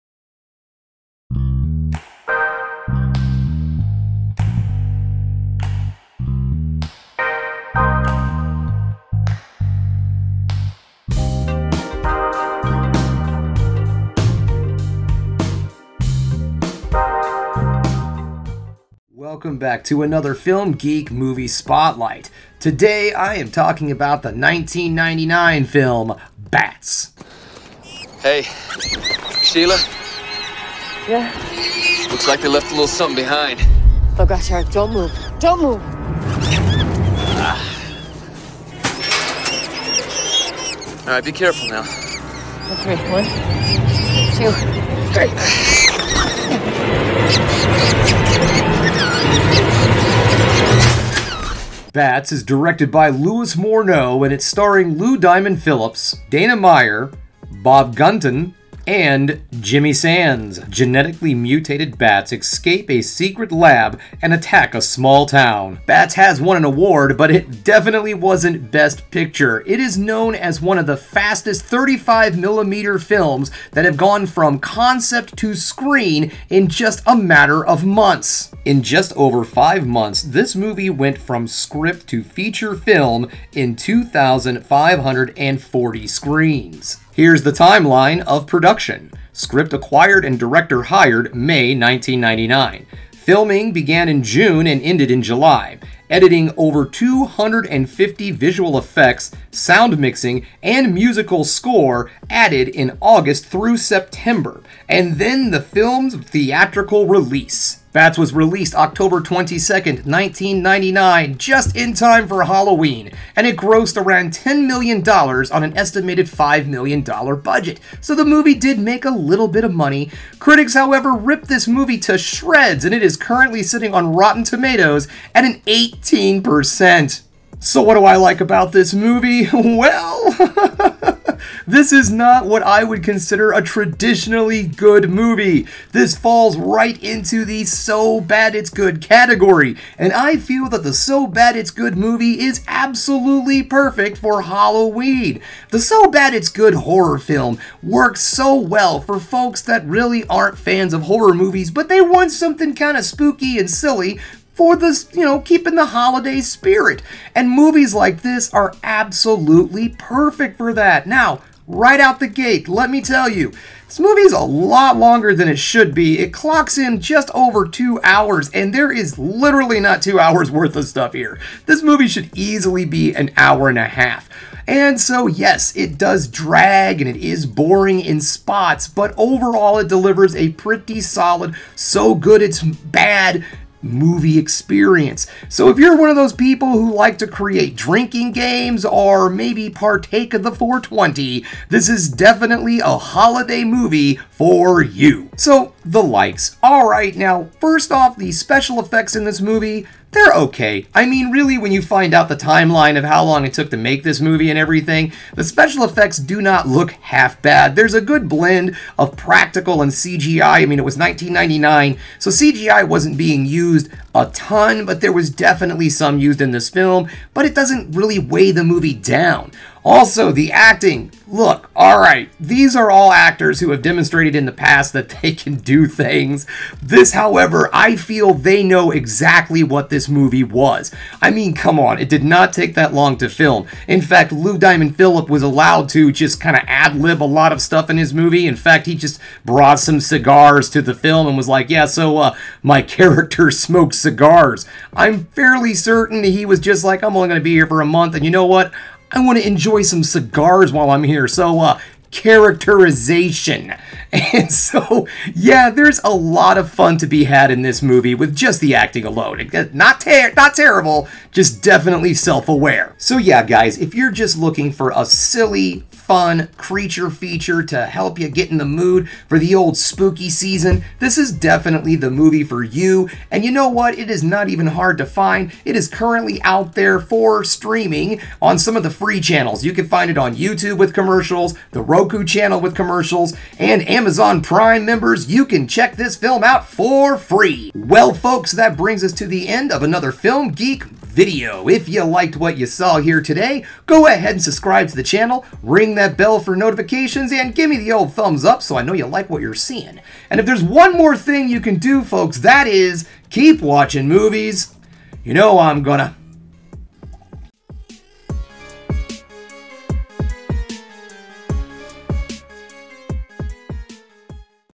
Bats (1999) Movie Spotlight
Spooky season continues with another Film Geek Movie Spotlight! Today I'm talking about the So Bad It's Good Movie "Bats" from 1999.